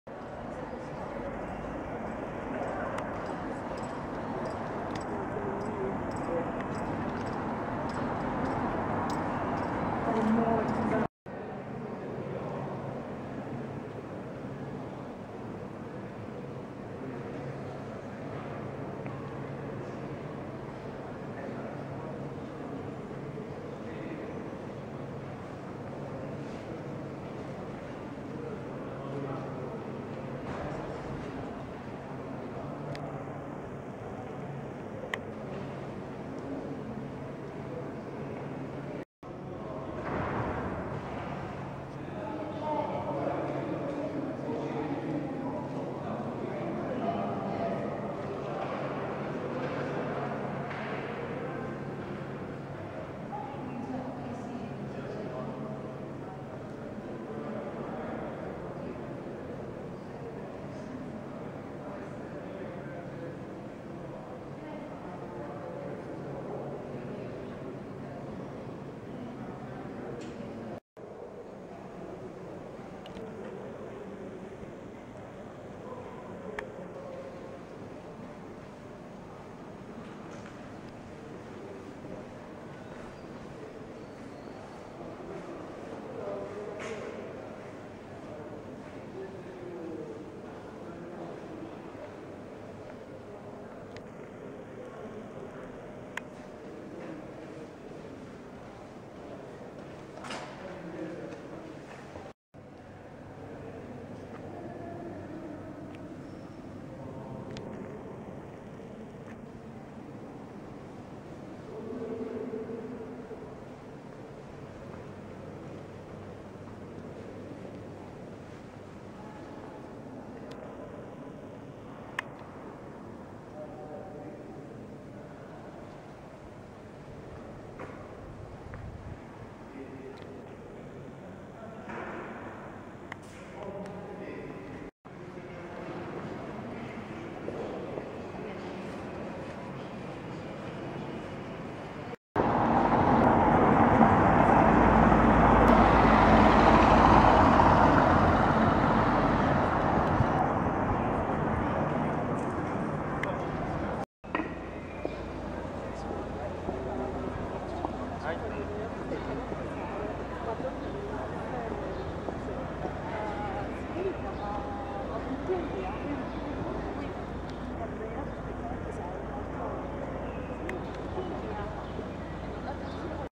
Pinacoteca di Brera - Milano, Italia
The Pinacoteca di Brera ("Brera Art Gallery") is the main public gallery for paintings in Milan, Italy. It contains one of the foremost collections of Italian paintings, an outgrowth of the cultural program of the Brera Academy, which shares the site in the Palazzo Brera.